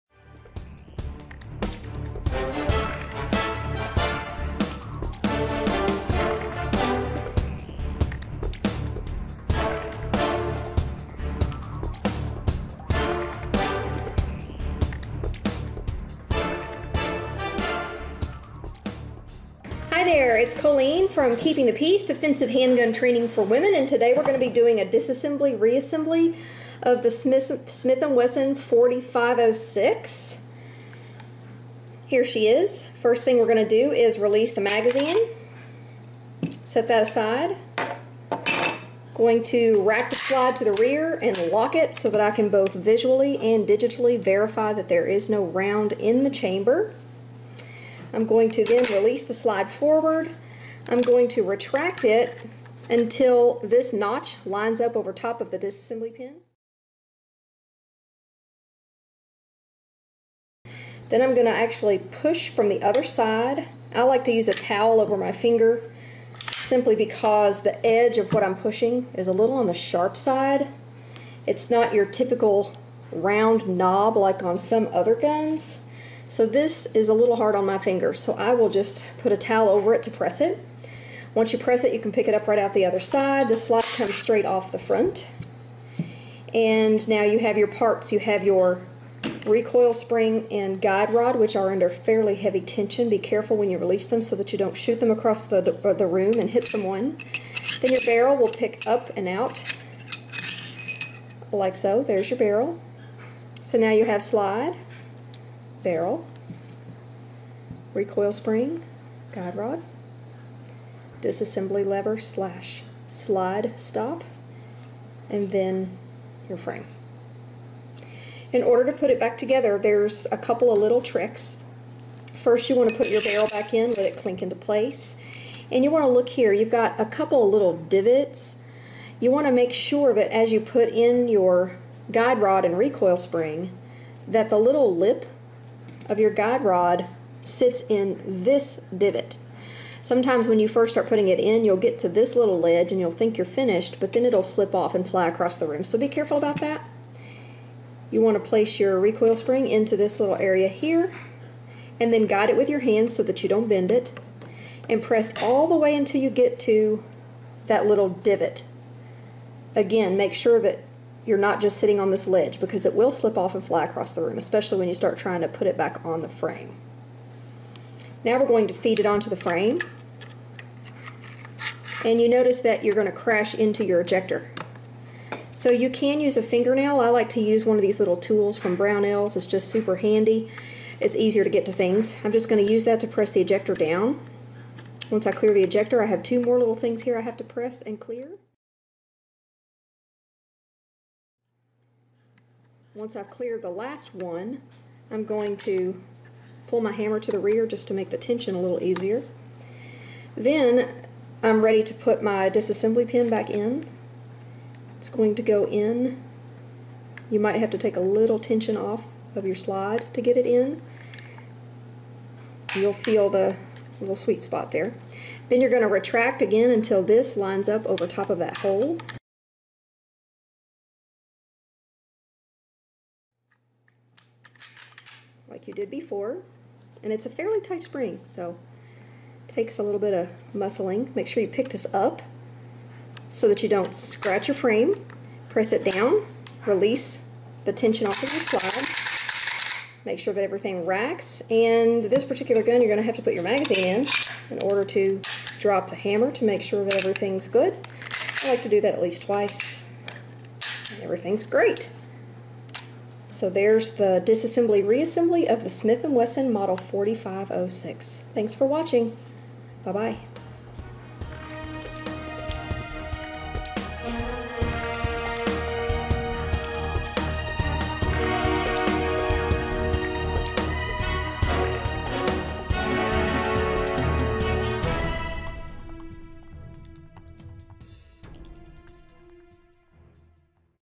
Smith & Wesson 4506 Disassembly & Reassembly
This video is a simple walk-through of the disassembly and reassembly process for the S&W 4506.